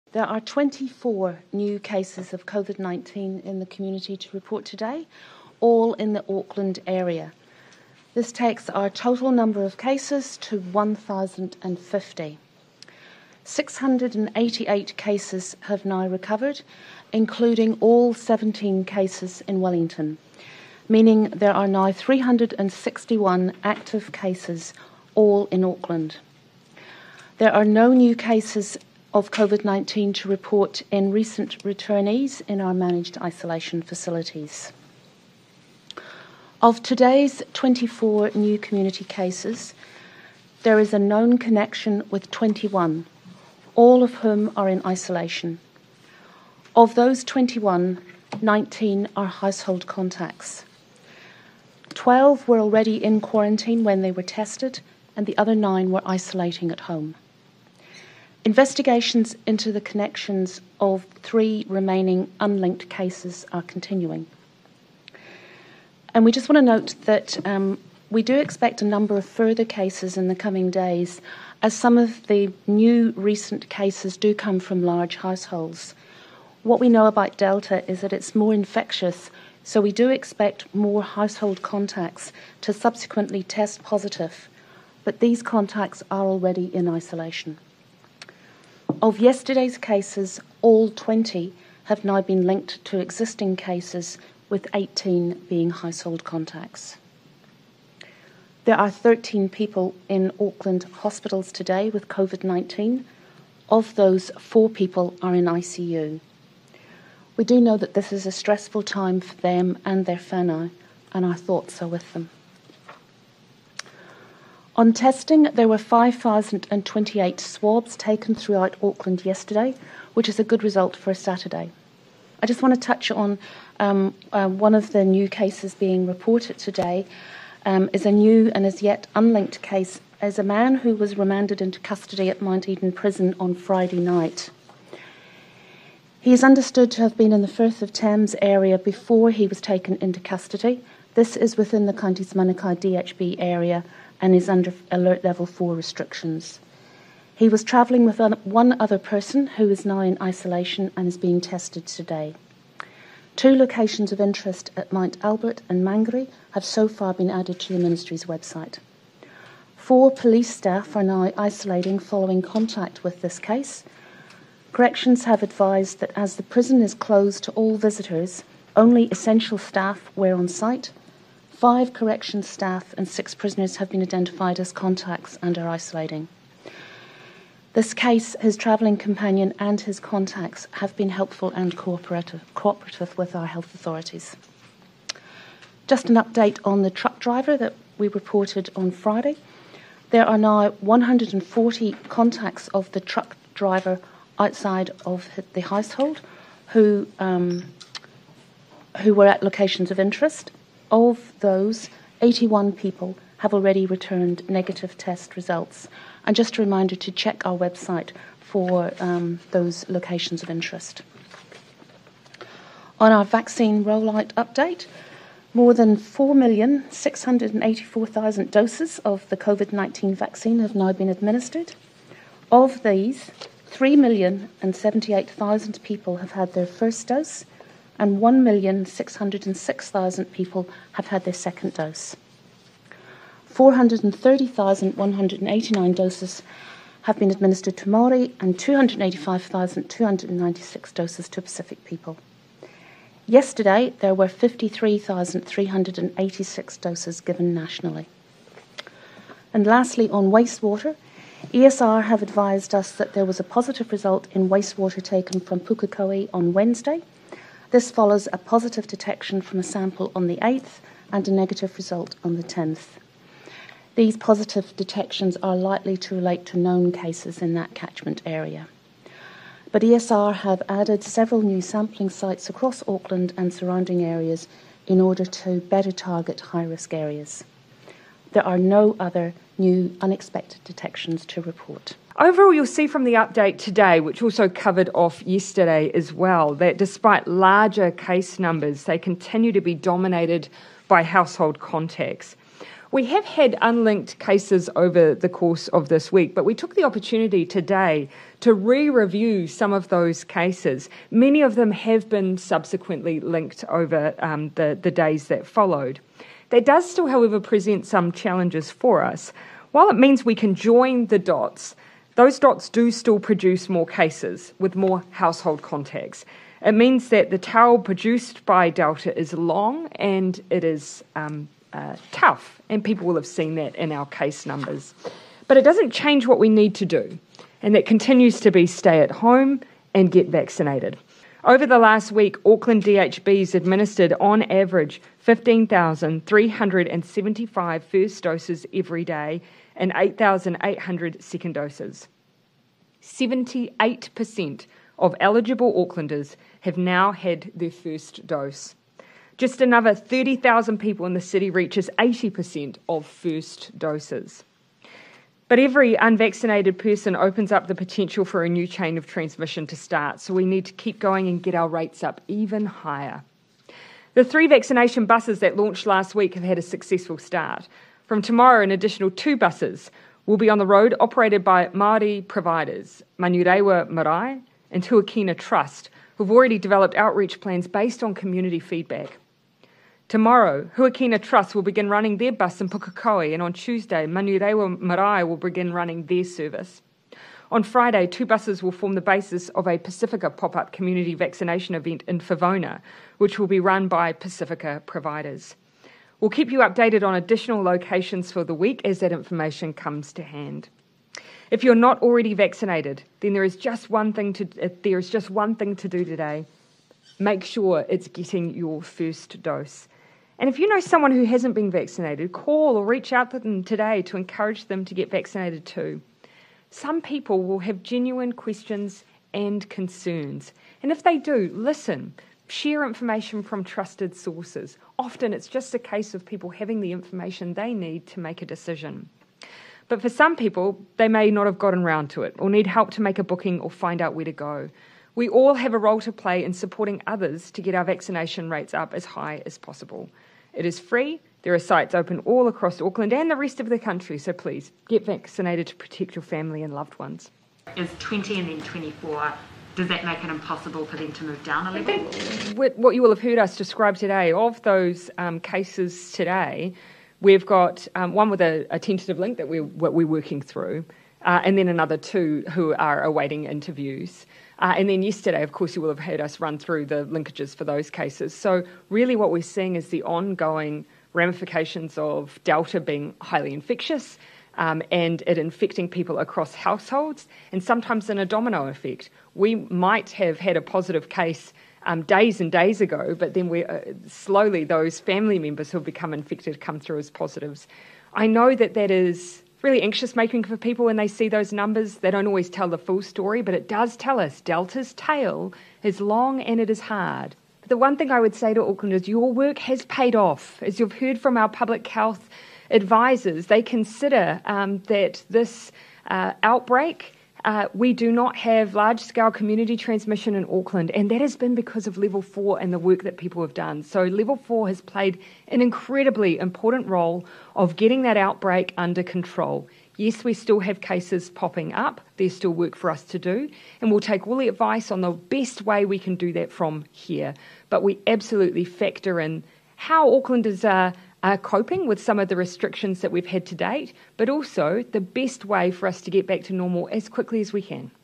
Covid-19: 24 new community cases - 3 unlinked to outbreak
There are 24 new Covid cases in the community in what is the penultimate day before Monday's big alert levels decision.